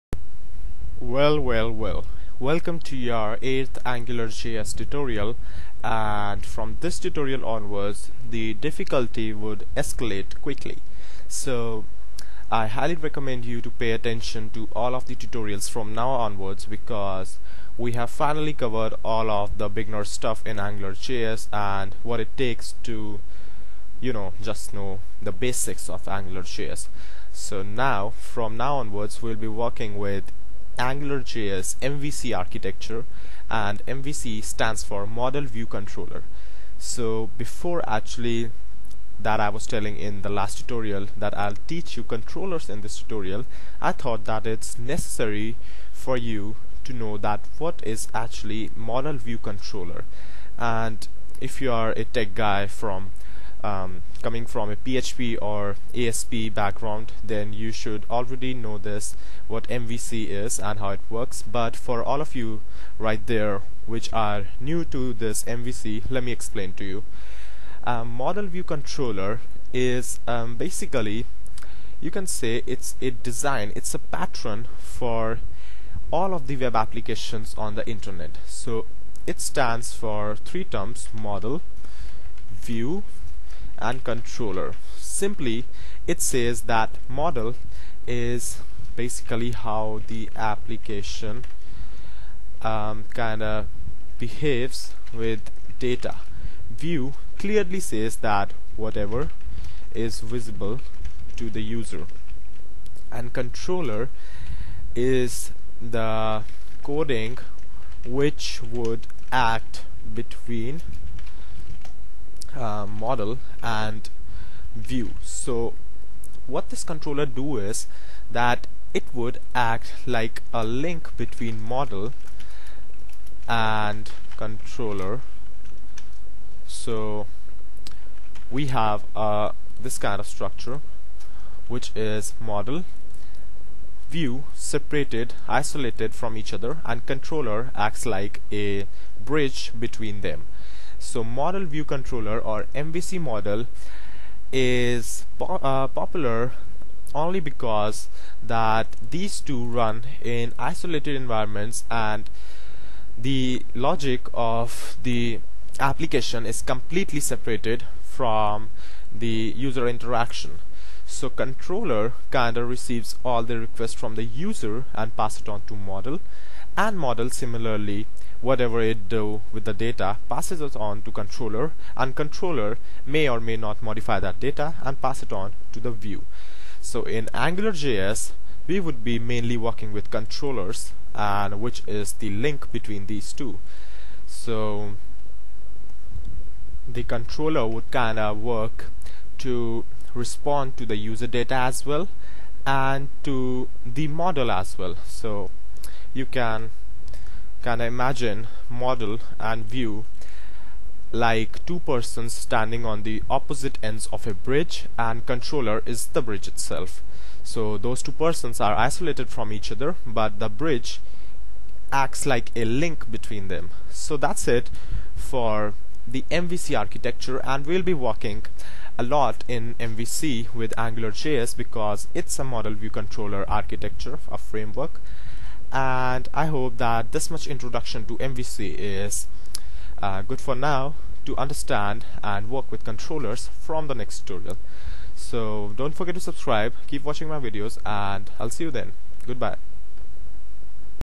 AngularJS Tutorial 8: Introduction To Model-View-Controller (MVC)
Learn what is MVC (model-view-controller) in AngularJS and how its helpful.

codedamn is a now free platform for developers to learn and establish their credibility. Ready to be among developers? Here you go